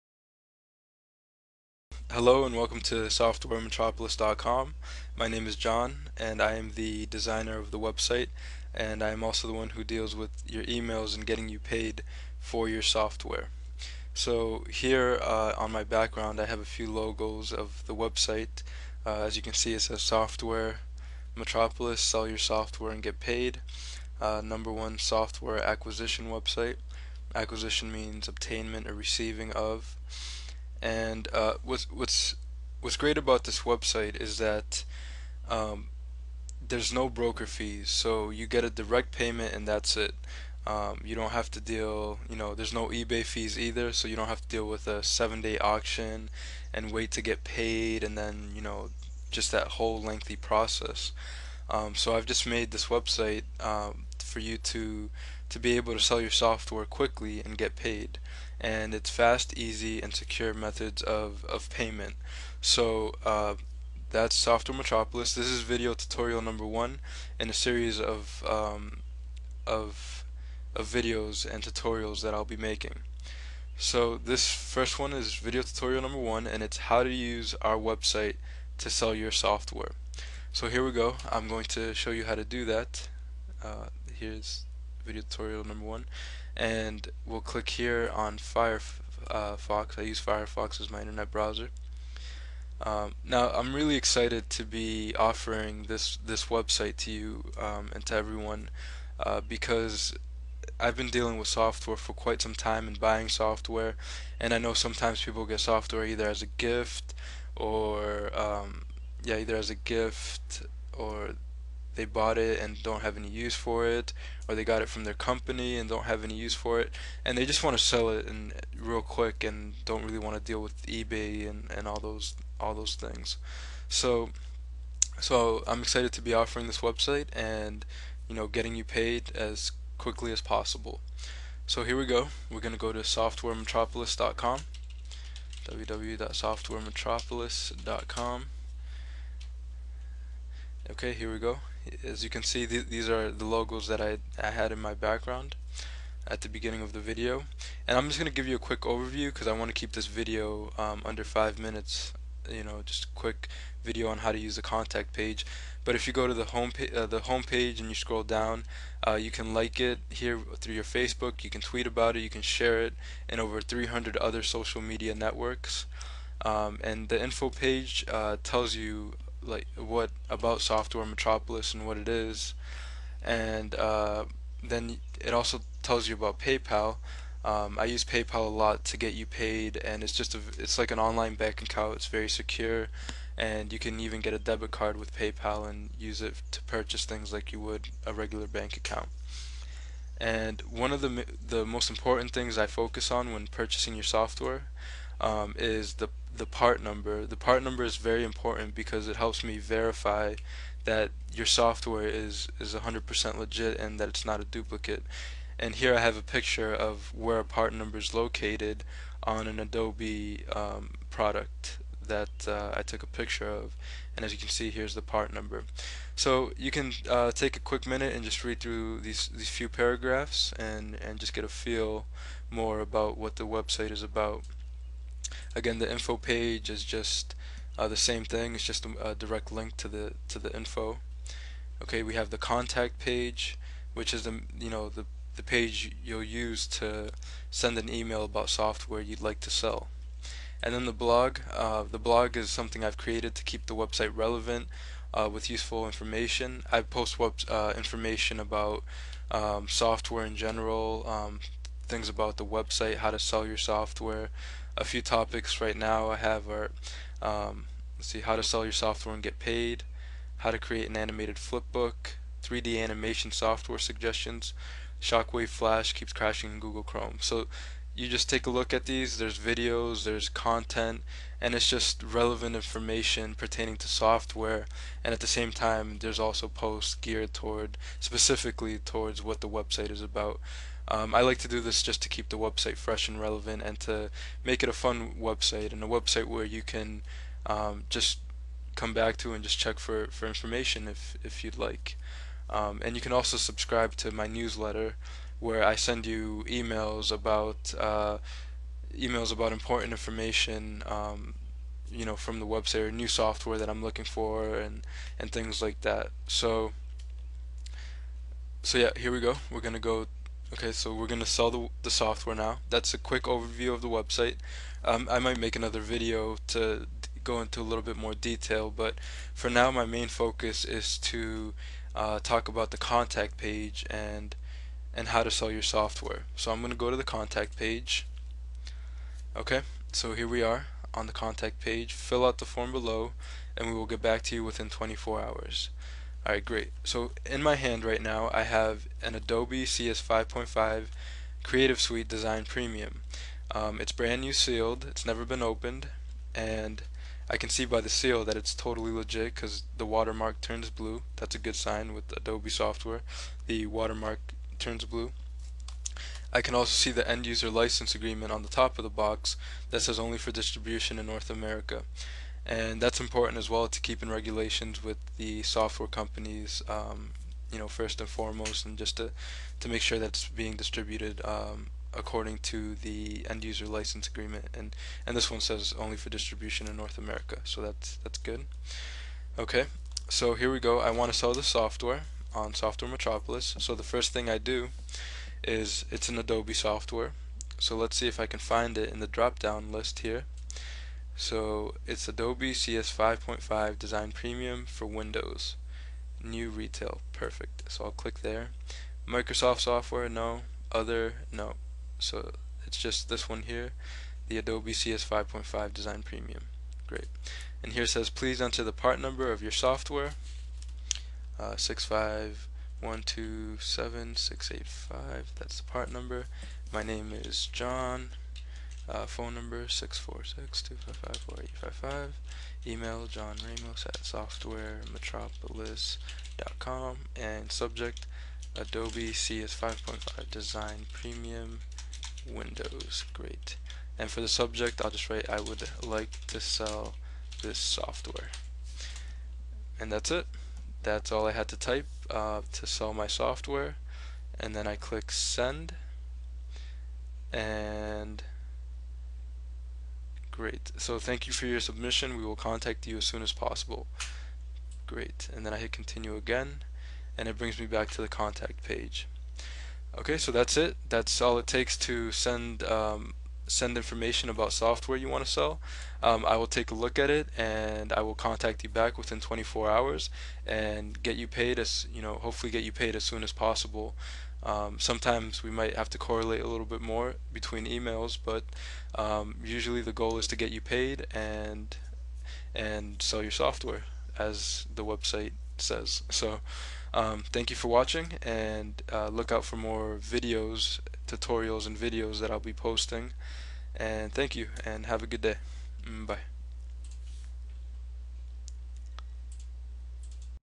How To Sell Your Software and Get Paid - Software Metropolis
In this video I show you how to sell your new or unwanted software and get paid via the